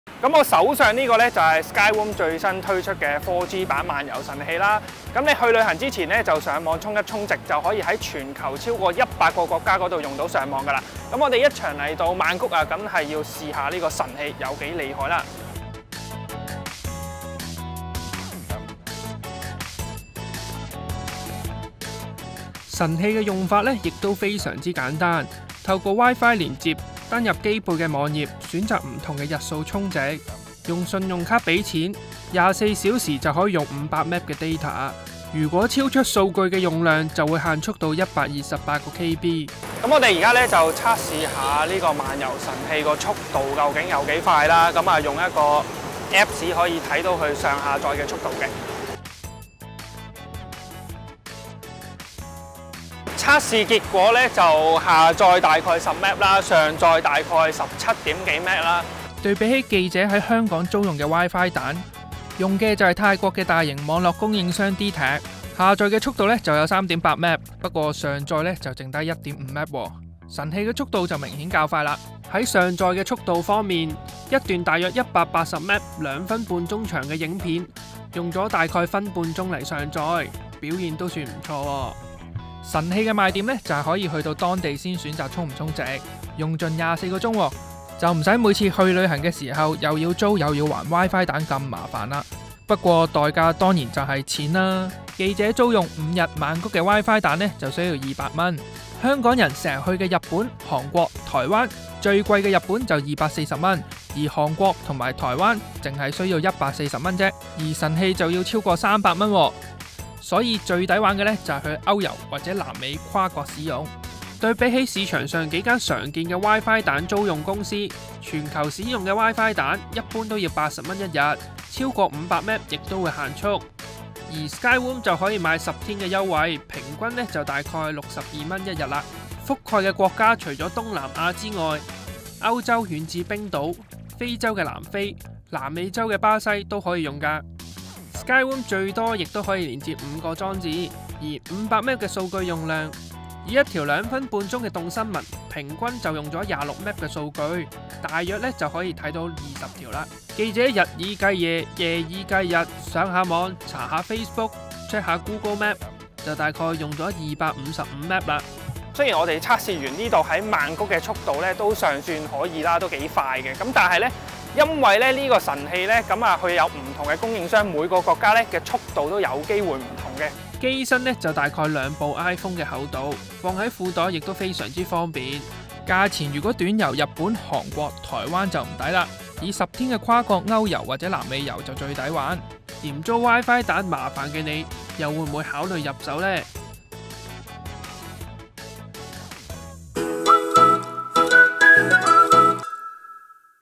實測漫遊神器隨時充值 最平每日$62過百國家用得
Skyroam最近推出的4G版漫遊神器，賣點是去旅行前可以選擇上網充值，就可以在全球超過100個國家使用上網，記者今日來到曼谷亦實試神器的威力。